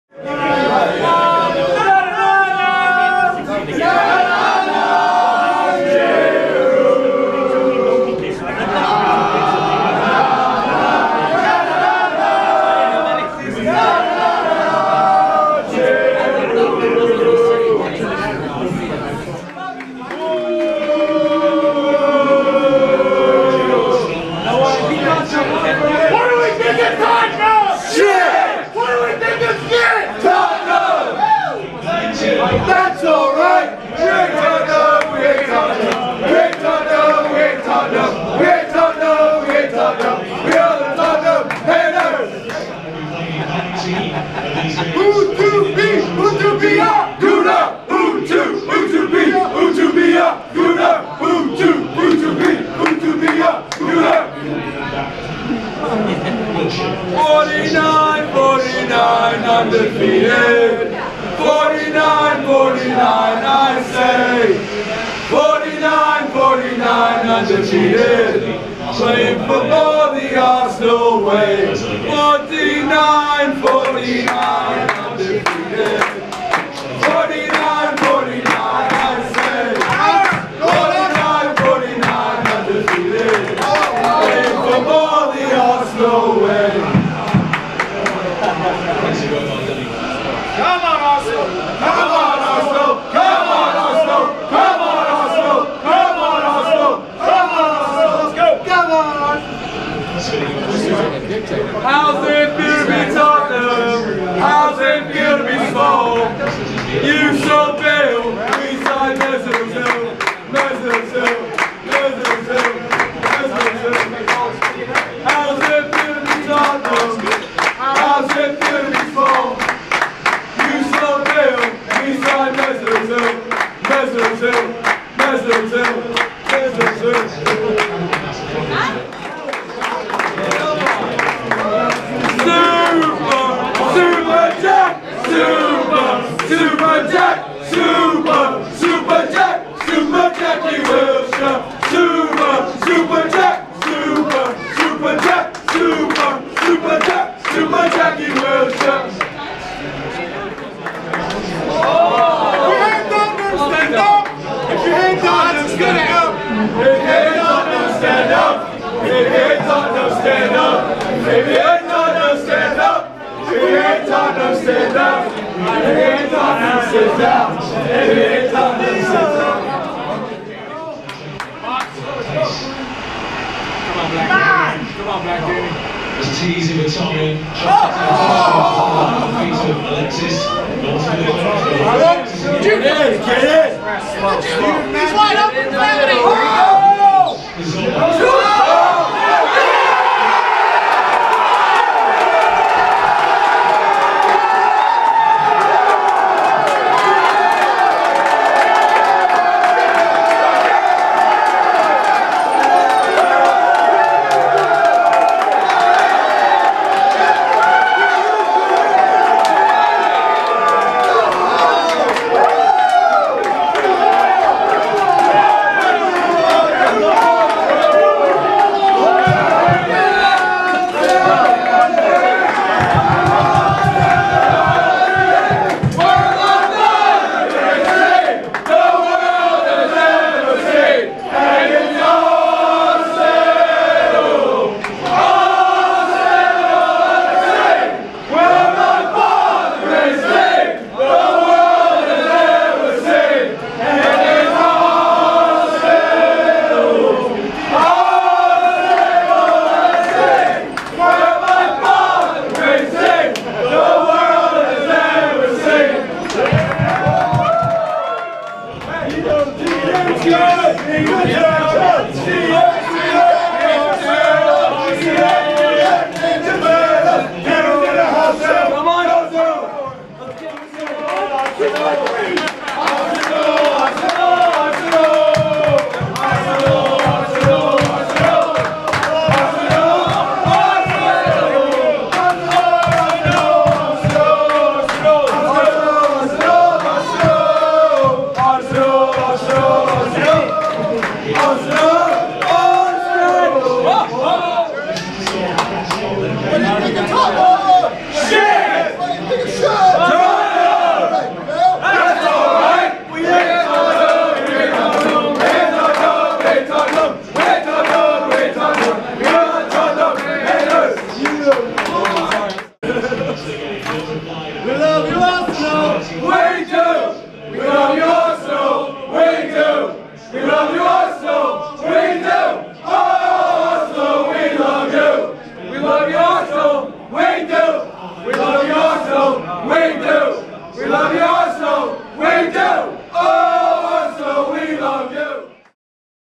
Arsenal v Tottenham - Septemeber 27, 2014
Arsenal v Tottenham @ Emirates Stadium (1 - 1)

Orlando Gooners @ XL Indoor Soccer World

North London Derby Meet Up

Audio not safe for little ears